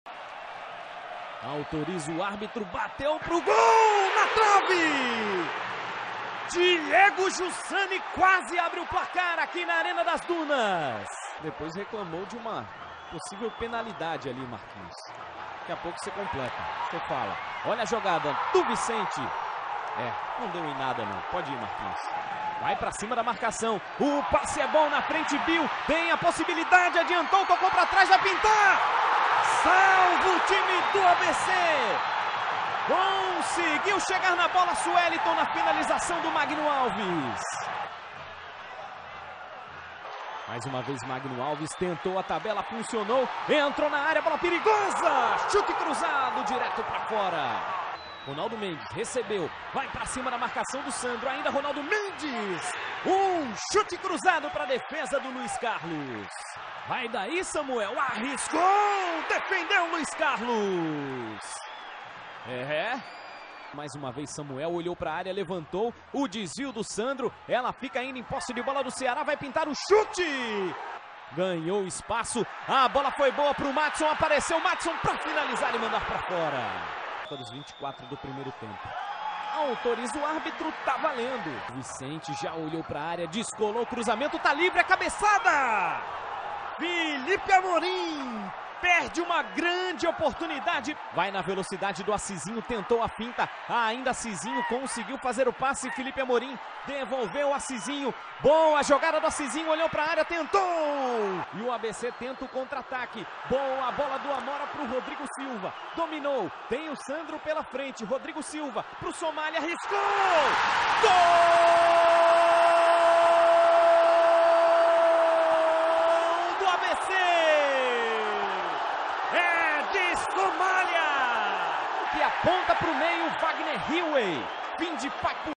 36ª Rodada - ABC 1 x 0 Ceará/CE
Confira os melhores momentos da partida do Mais Querido contra o Ceará.